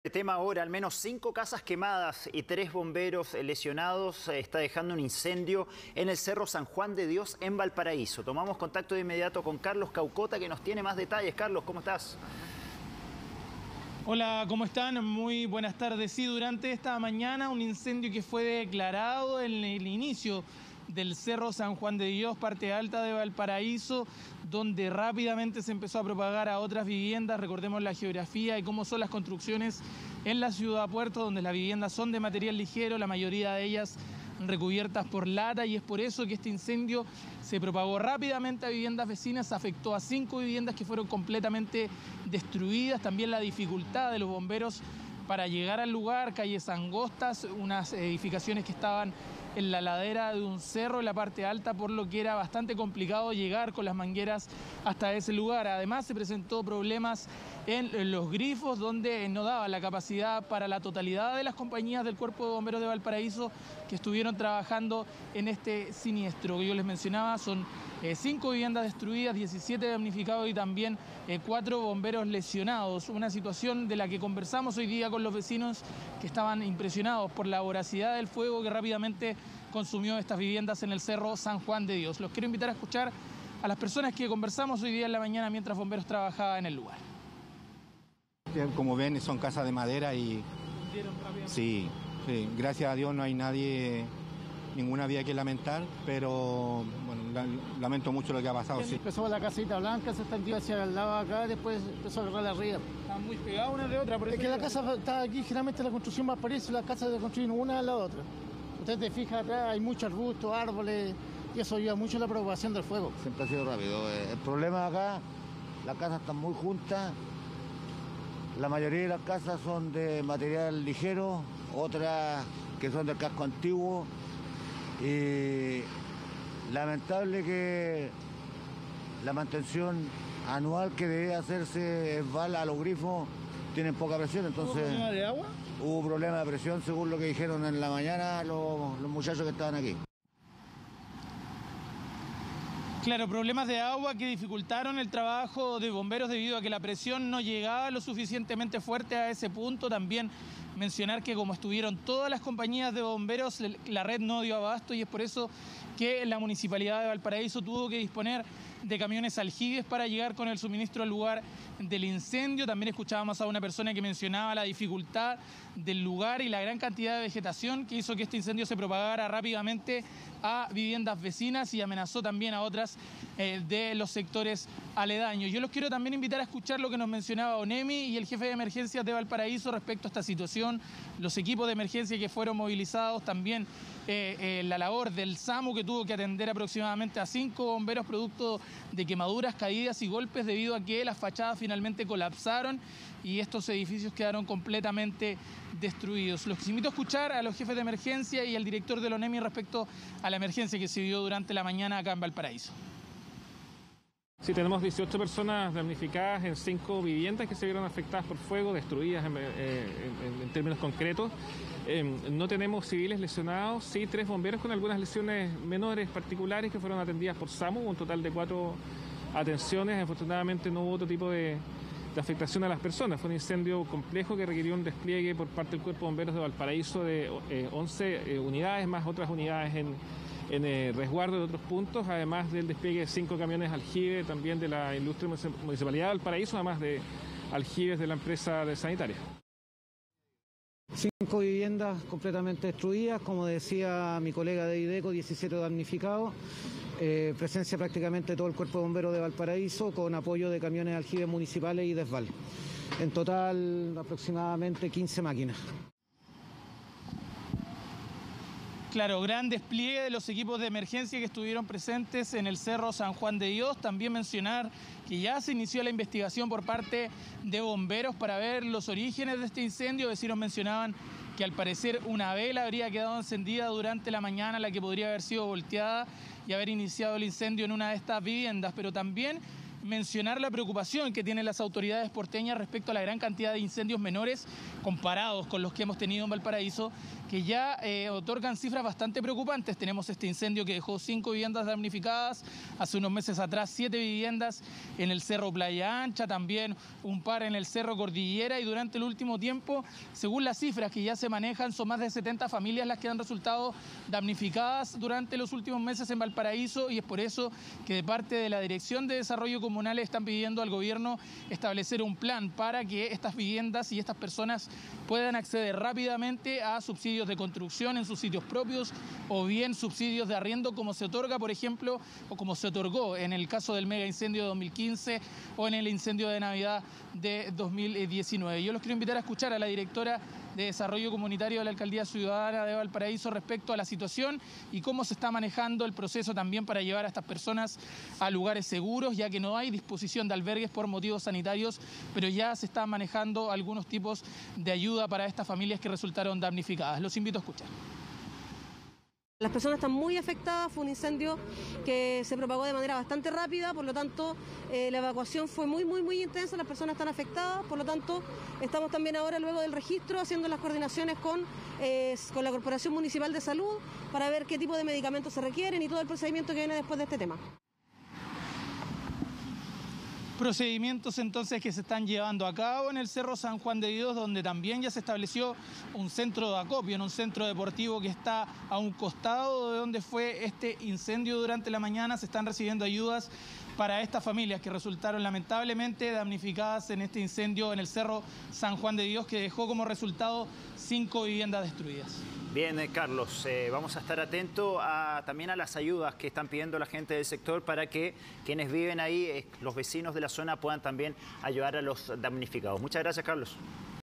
Incendio consume al menos cinco casas en cerro San José de Dios en Valparaíso
El siniestro comenzó por motivos que se están investigando, pero personal de Bomberos de la región acudió rápidamente hasta el sector.